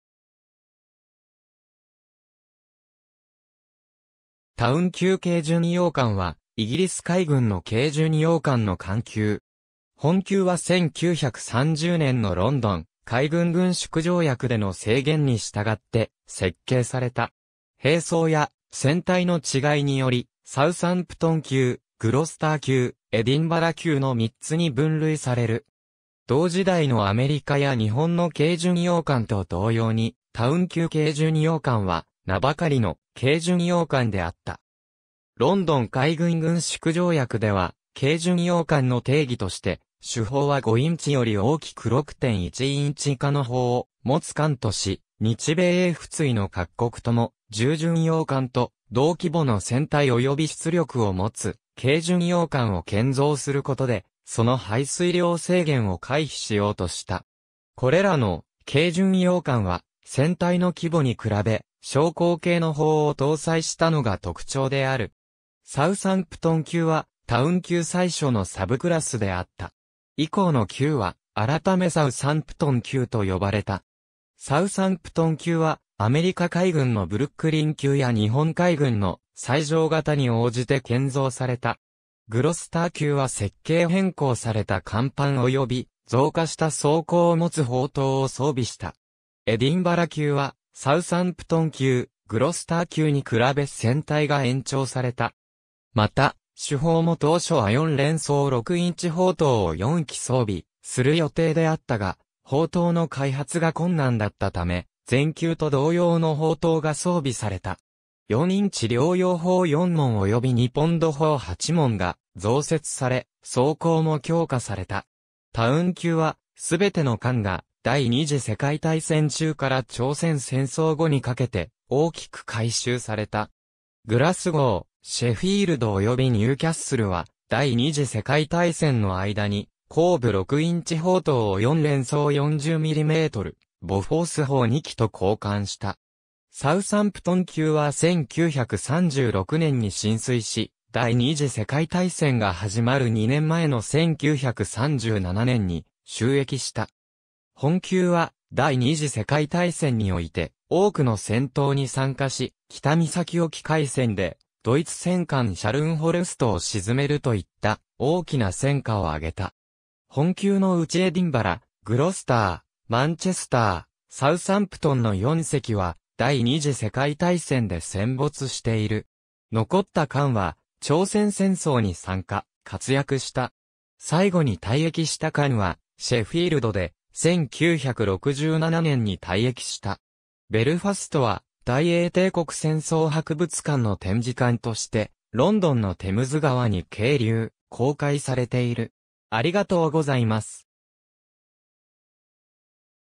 タウン級軽巡洋艦 (2代)
タウン級軽巡洋艦 (Town-class) は、イギリス海軍の軽巡洋艦の艦級。
本級は1930年のロンドン海軍軍縮条約での制限に従って設計された。
兵装や船体の違いにより、サウサンプトン級、グロスター級、エディンバラ級の三つに分類される。
同時代のアメリカや日本の軽巡洋艦と同様に、タウン級軽巡洋艦は名ばかりの「軽巡洋艦」であった。
ロンドン海軍軍縮条約では「軽巡洋艦」の定義として、主砲は5インチより大きく6.1インチ (155 mm) 以下の砲を持つ艦とし、日米英仏伊の各国とも、重巡洋艦と同規模の船体および出力を持つ軽巡洋艦を建造することで、その排水量制限を回避しようとした。
これらの「軽巡洋艦」は船体の規模に比べ小口径の砲を搭載したのが特徴である。
サウサンプトン級はタウン級最初のサブクラスであった。
以降の級は改サウサンプトン級と呼ばれた。
サウサンプトン級はアメリカ海軍のブルックリン級や日本海軍の最上型に応じて建造された。
グロスター級は設計変更された甲板および増加した装甲を持つ砲塔を装備した。
エディンバラ級はサウサンプトン級、グロスター級に比べ船体が延長された。
また、主砲も当初は4連装6インチ砲塔を4基装備する予定であったが、砲塔の開発が困難だったため、前級と同様の砲塔が装備された。
4インチ両用砲4門および2ポンド砲8門が増設され、装甲も強化された。
タウン級はすべての艦が第二次世界大戦中から朝鮮戦争後にかけて大きく改修された。
グラスゴー、シェフィールドおよびニューキャッスルは第二次世界大戦の間に後部6インチ砲塔を4連装40mmボフォース砲2基と交換した。
サウサンプトン級は1936年に進水し、第二次世界大戦が始まる2年前の1937年に就役した。
本級は第二次世界大戦において多くの戦闘に参加し、北岬沖海戦でドイツ戦艦シャルンホルストを沈めると言った大きな戦果を挙げた。
本級の内エディンバラ、グロスター、マンチェスター、サウサンプトンの4隻は第二次世界大戦で戦没している。
残った艦は朝鮮戦争に参加、活躍した。
最後に退役した艦はシェフィールドで、1967年に退役した。
ベルファストは大英帝国戦争博物館の展示艦としてロンドンのテムズ川に係留、公開されている。